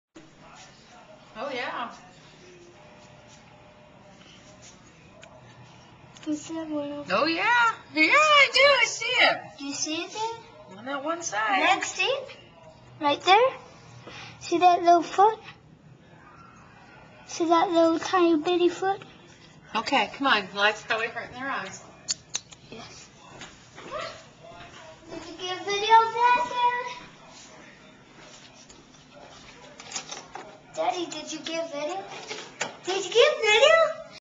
Tadpoles To Frogs - Maybe?
Since April 2009, these two tadpoles have been living it easy in Washington. frogs may arrive soon.